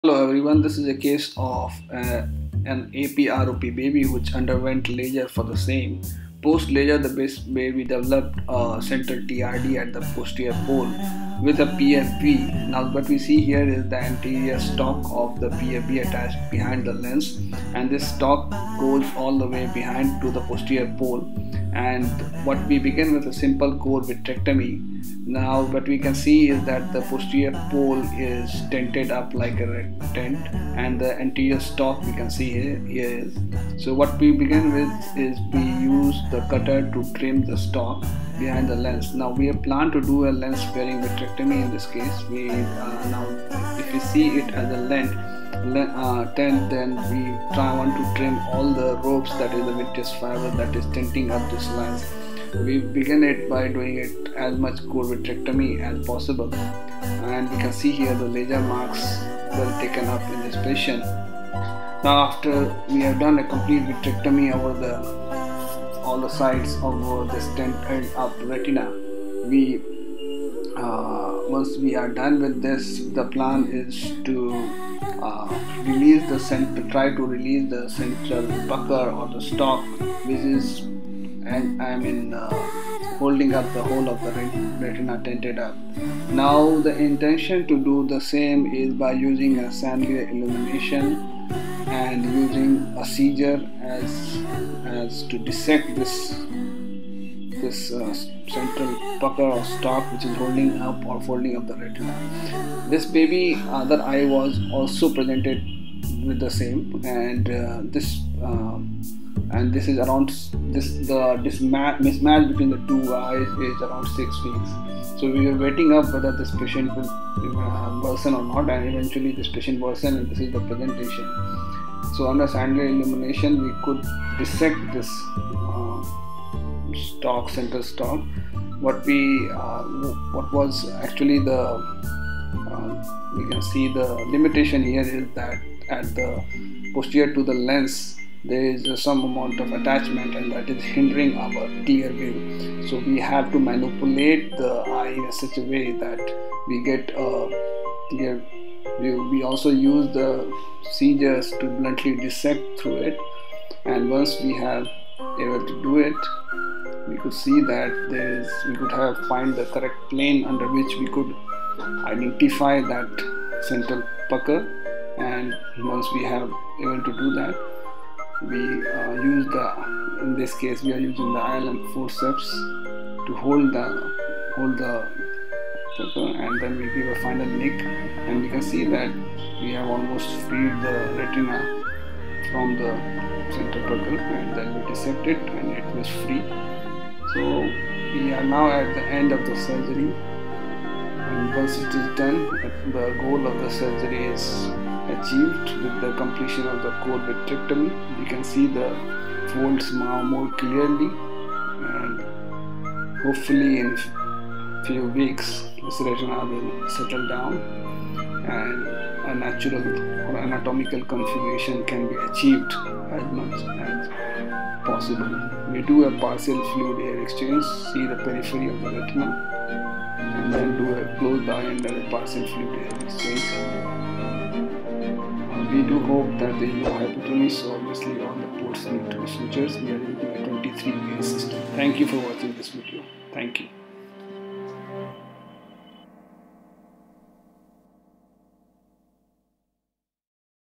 ROP WITH PFV
22 week old baby post LASER PRP for Aggressive ROP  developed a central TRD with Persistent Fetal Vasculature. Patient was planned for Lens Sparing vitrectomy along with Removal of Fibrovascular Stalk